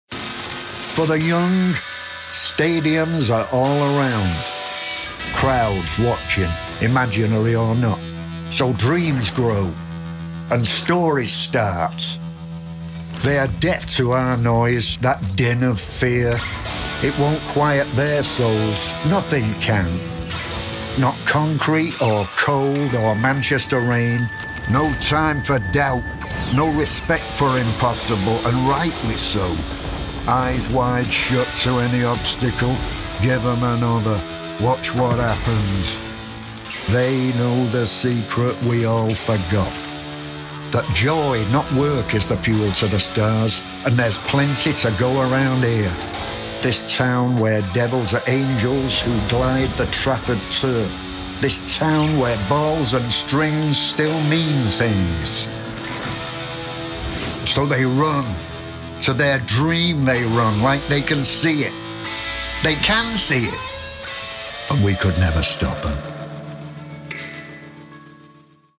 JUST DROPPED: THE MANCHESTER UNITED 2021/22 AWAY KIT
Manchester United and adidas reveal today the new 2021/22 Away kit, inspired by the iconic design of ’92. Narrated by Dr. John Cooper Clarke, this kit draws inspiration from the collective memory and DNA of the club through a new and modern expression. It’s inspired by one of the most recognisable jerseys in the club’s history.